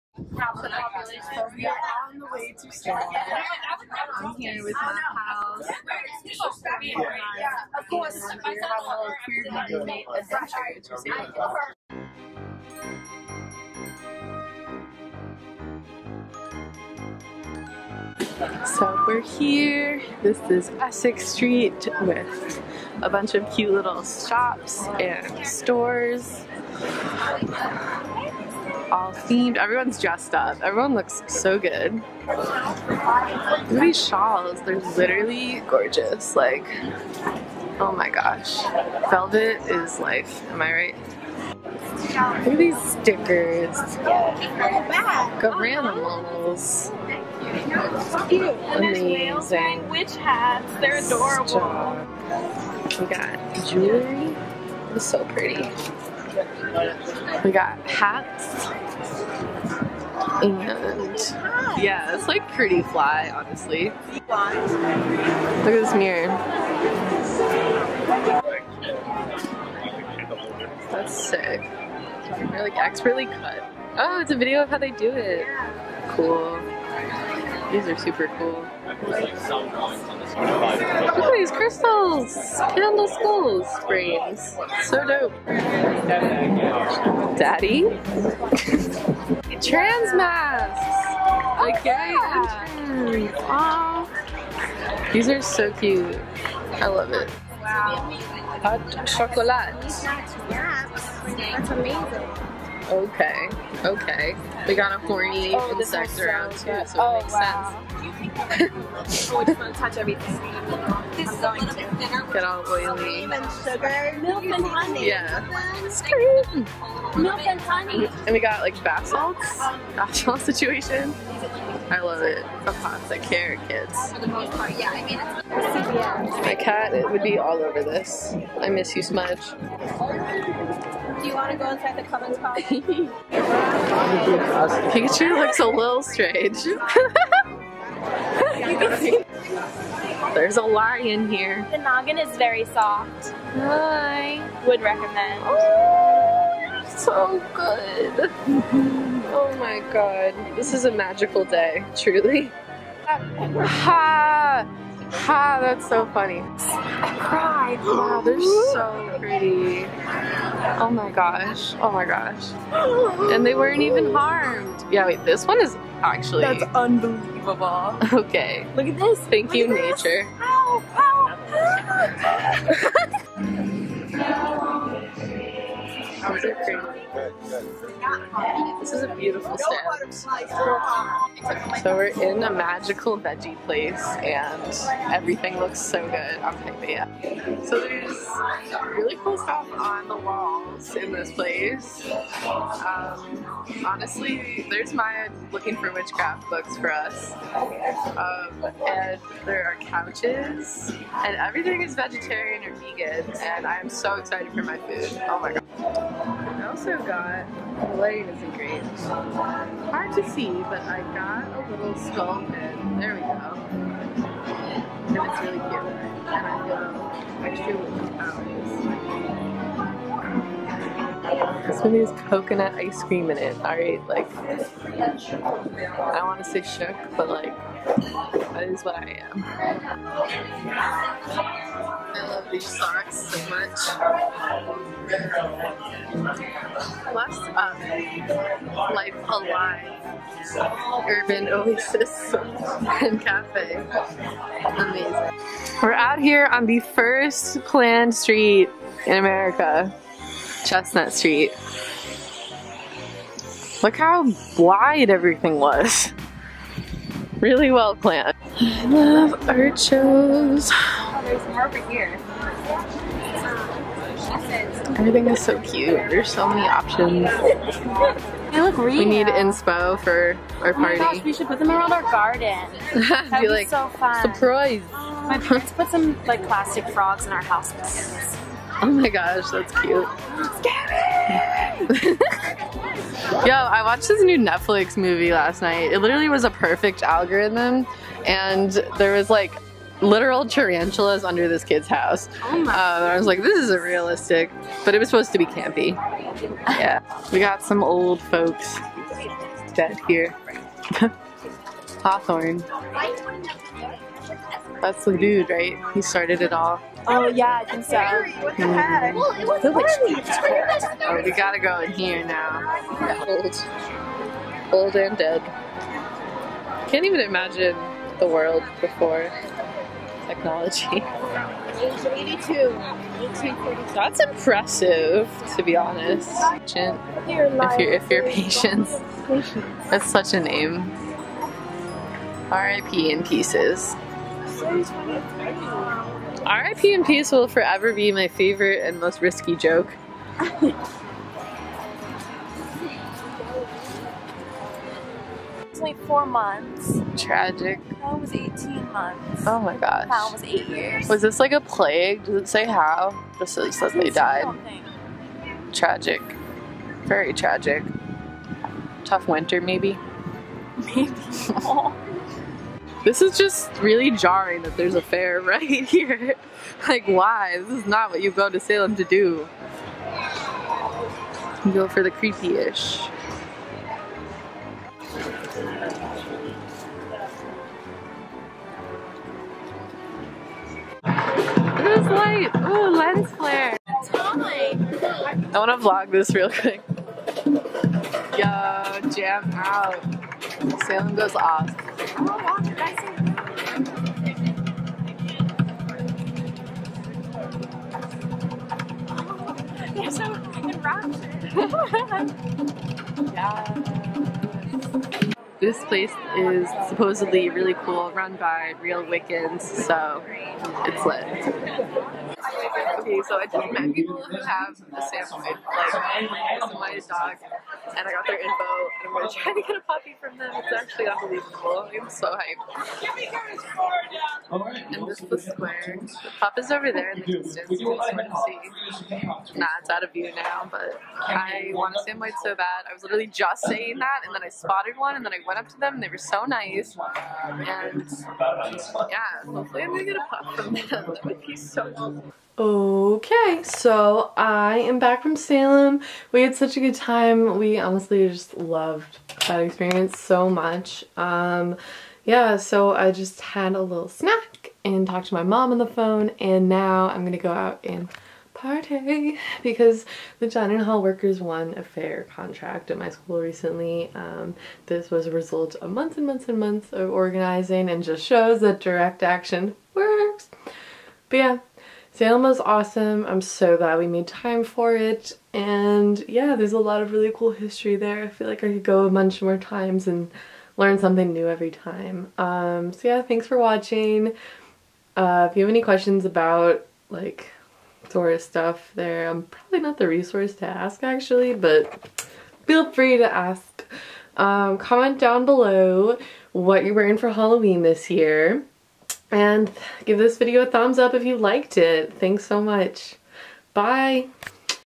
Spooky Salem Witches Adventure Vlog! Happy Halloween!!!
Today we decided to take a quick weekend trip to Salem, MA, which is only 30 minutes north of Boston. It's safe to say that the place is bursting with history and fun!!!

Sounds too good to be true but isn't:
Use my Postmates code for $100 of free delivery!!! MIEB2